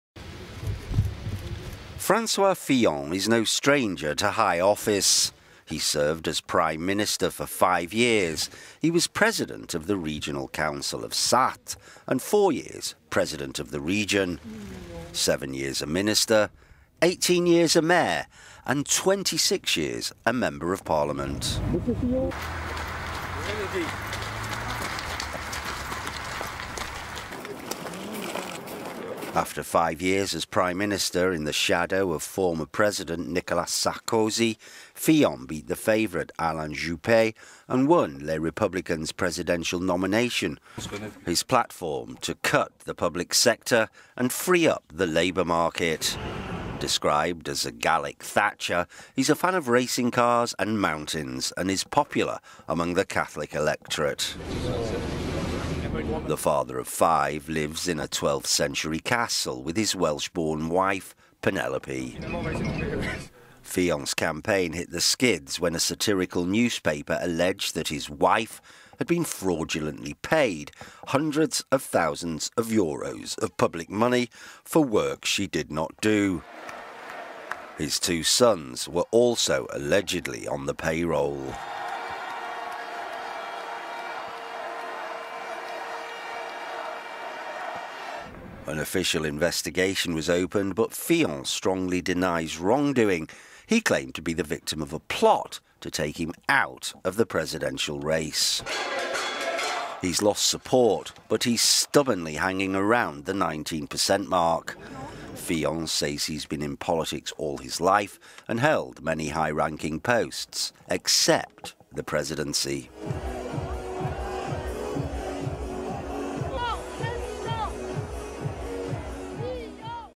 Fillon and the perils of Penelope
François Fillon is no stranger to high office. He served as prime minister for five years, he was president of the
regional council of Sarthe, four years president of the region, seven years a minister, eighteen years mayor and 26 years a member of parliament.

LIVE: French presidential candidate Francois Fillon makes a statement after the Paris attack Reuters World (@ReutersWorld) April 21, 2017